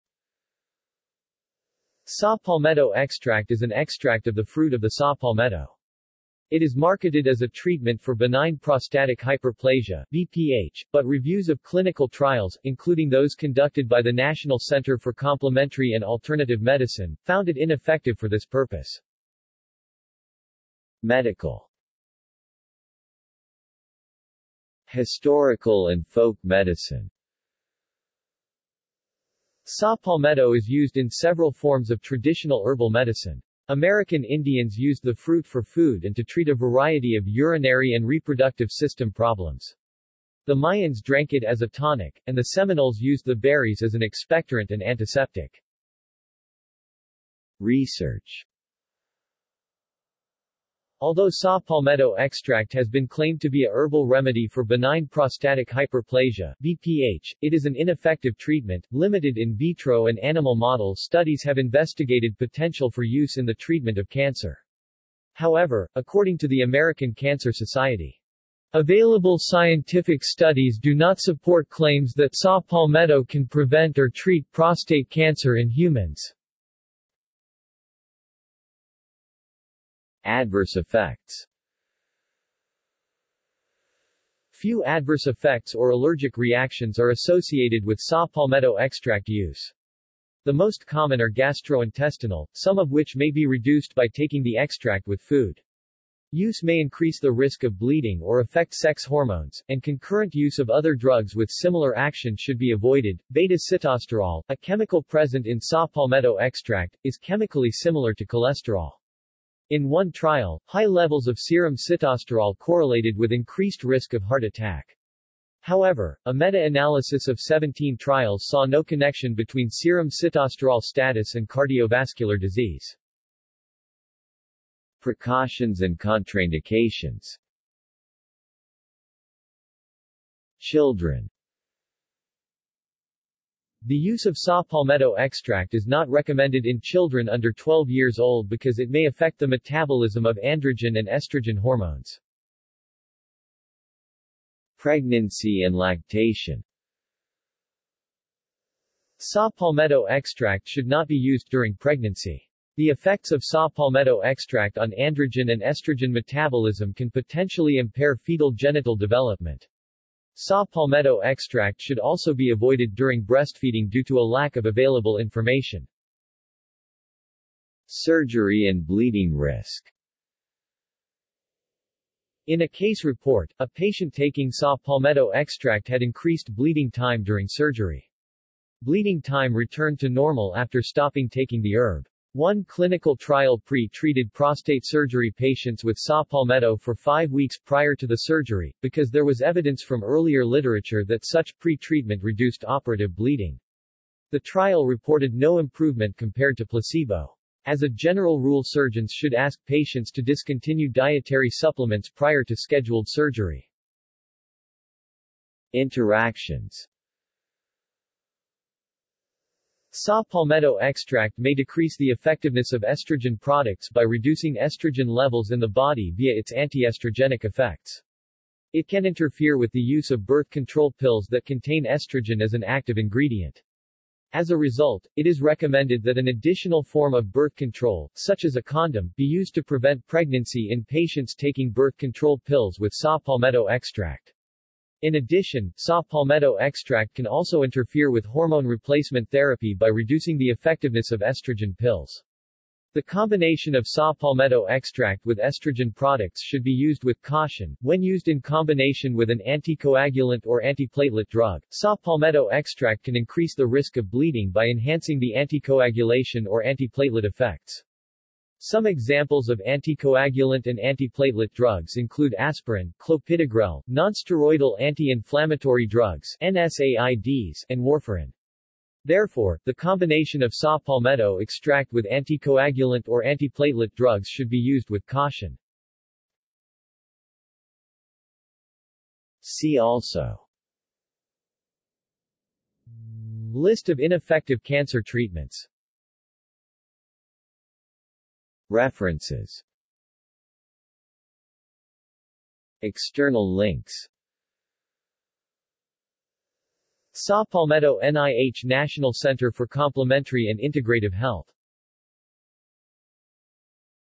Saw palmetto extract | Wikipedia audio article
This is an audio version of the Wikipedia Article:
Saw palmetto extract

SUMMARY
Saw palmetto extract is an extract of the fruit of the  saw palmetto. It is marketed as a treatment for benign prostatic hyperplasia (BPH), but reviews of clinical trials, including those conducted by the National Center for Complementary and Alternative Medicine, found it ineffective for this purpose.